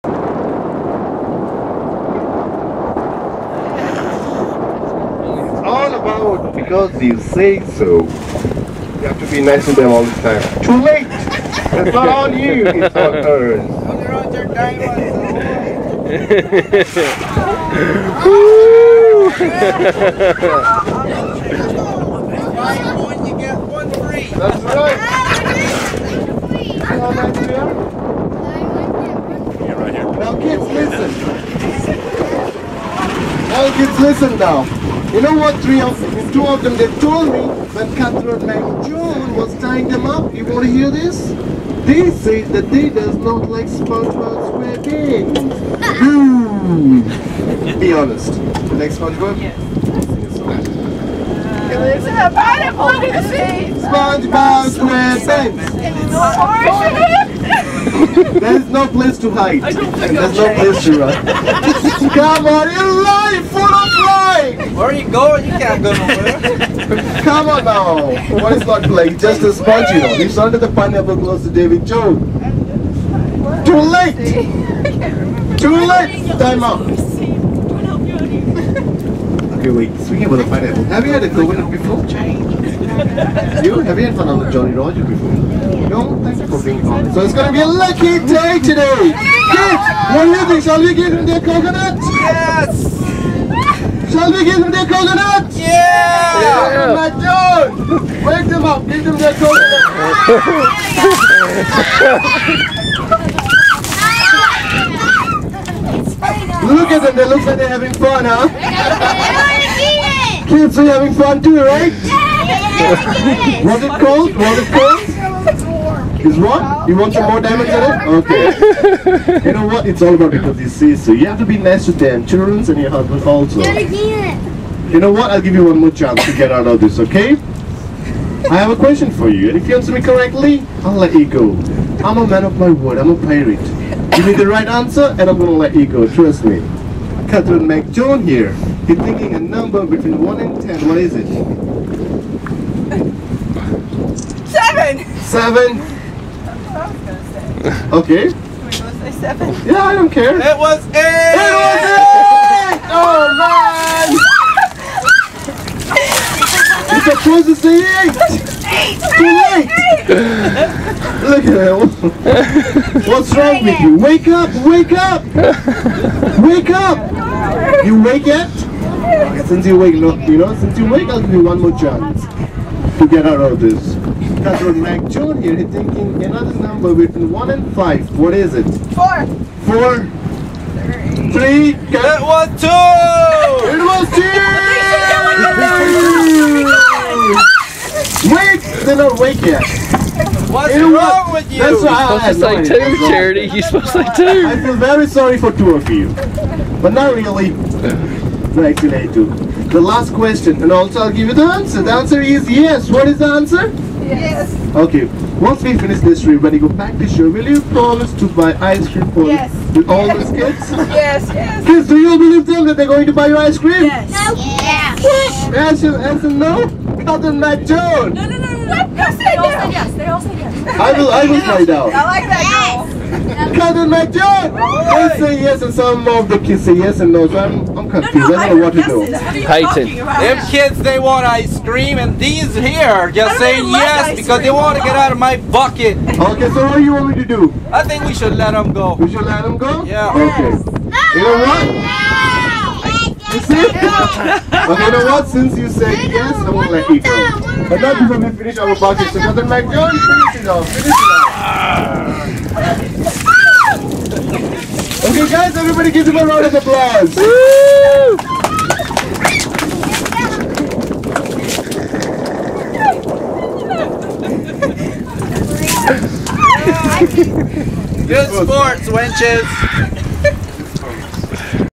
Pirate Attack!!
While we were on our cruise we went on a pirate ship. The pirates picked me and Faith out of the crowd and tied us up--Then ask us questions (that no one could get right) and every time we got one wrong, they poured ICE water on our heads! Mean Pirates!! LOL